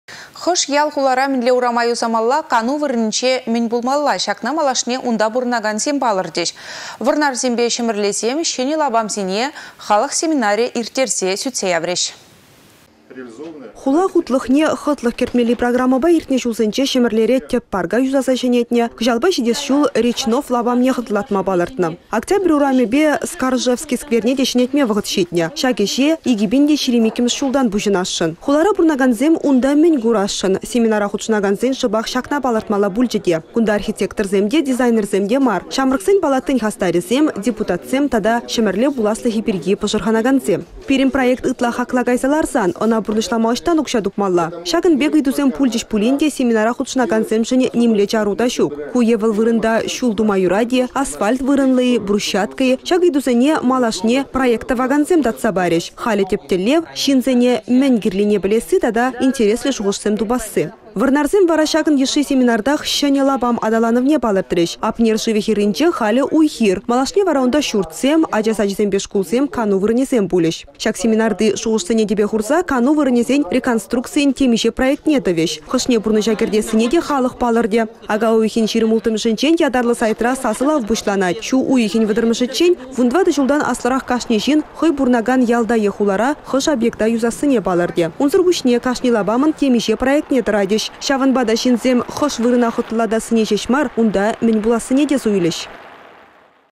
Мĕн юсассине халăх палăртĕ
Хăш ял-хулара мĕнле урама юсамалла, кану вырăнĕнче мĕн пулмалла — çакна малашне унта пурăнакансем палăртĕç. Вăрнарсемпе Çĕмĕрлесем çĕнĕ лапамсене  халăх семинарĕ ирттерсе сӳтсе яврĕç.